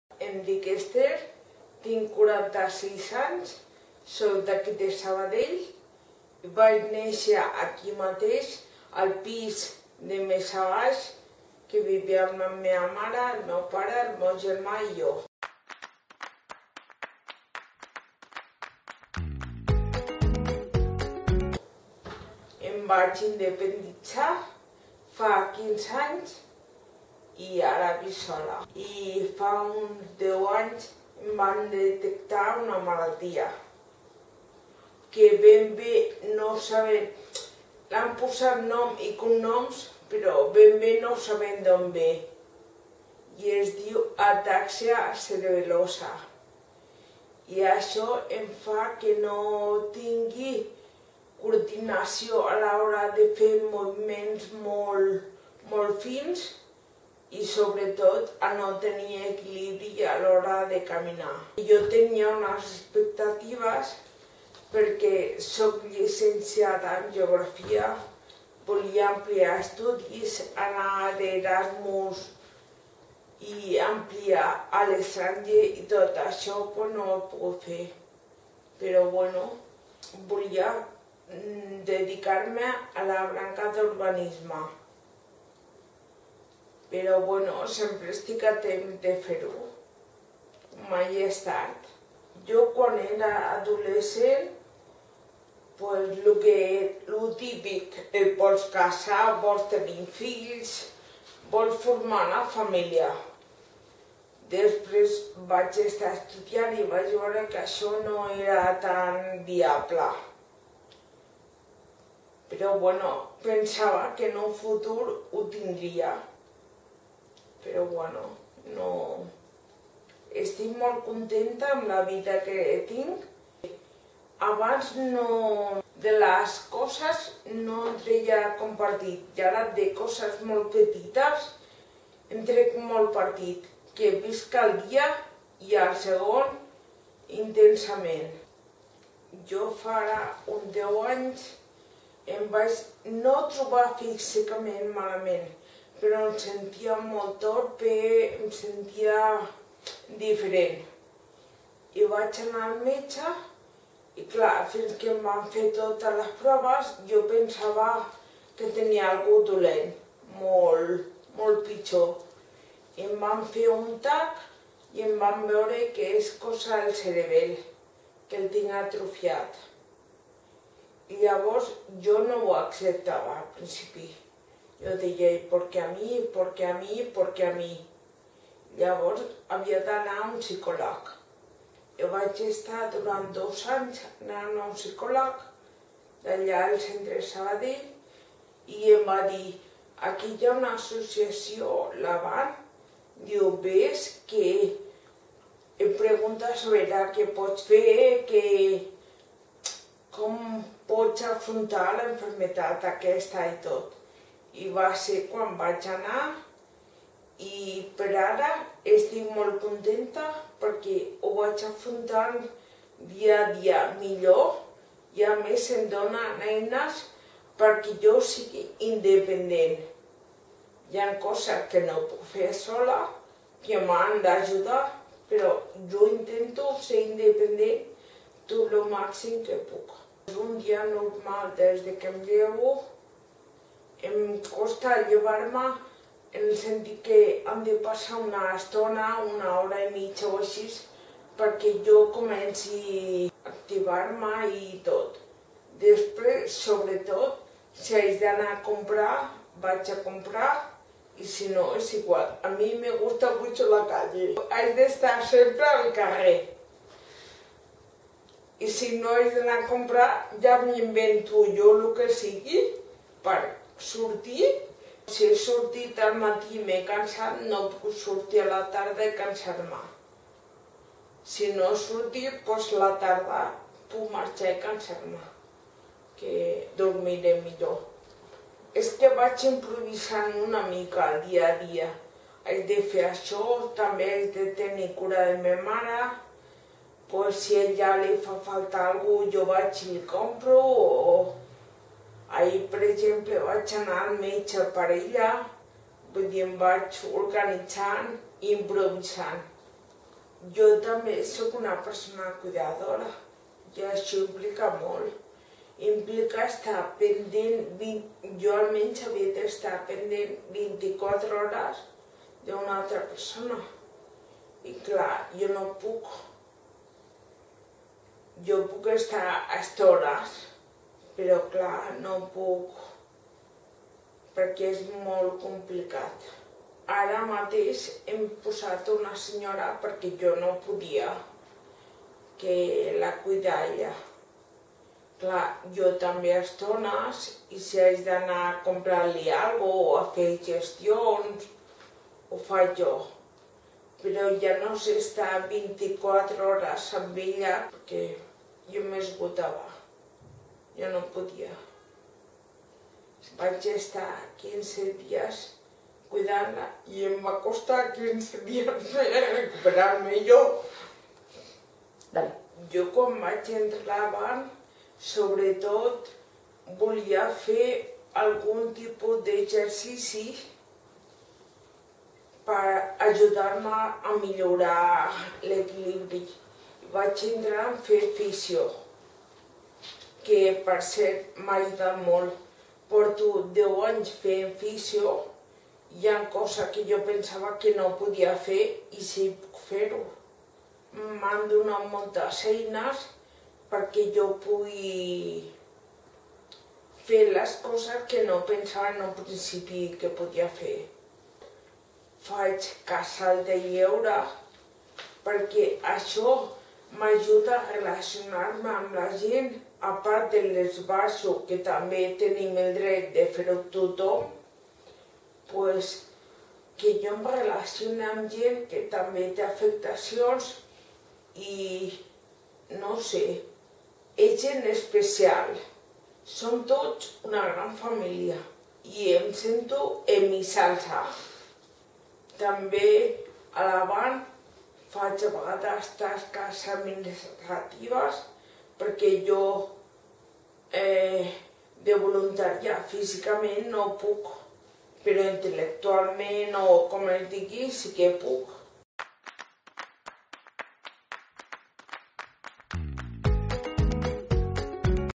JUNY 2022 - ESTHER SALLA
Ella és l'Esther Salla, voluntària i usuaria de l'AVAN des de fa més de deu anys! Ens encanta l'optimisme amb el què ens explica la seva història i el seu testimoni ens demostra que paset a paset podem arribar molt lluny!

Aquesta és una de les 12 històries protagonistes del nostre Calendari Solidari 2022, un projecte de la Fundació AVAN Neurologia que ens ajuda a captar donatius per tal de poder financiar la nostra tasca; oferir teràpies de neurorehabilitació a persones afectades per malalties neurològiques i a les seves famílies.